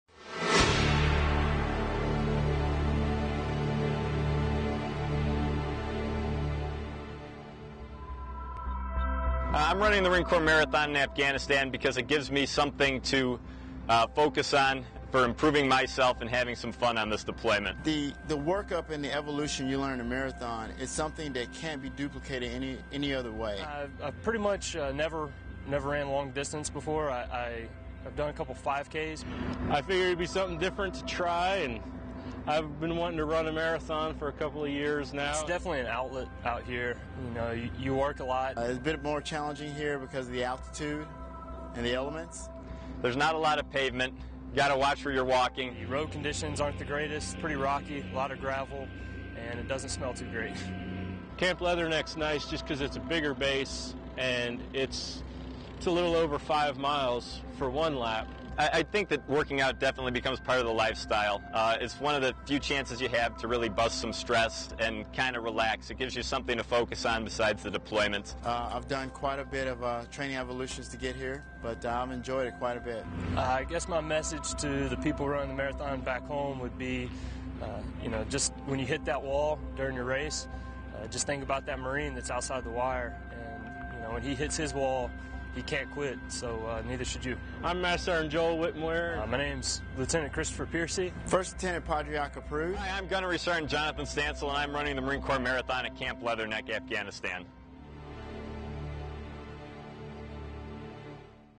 Camp Leatherneck Marines and service members run marathon
Service members at Camp Leatherneck, Afghanistan, take some time aside to challenge their physical strength and spirit by running the Marine Corps Marathon.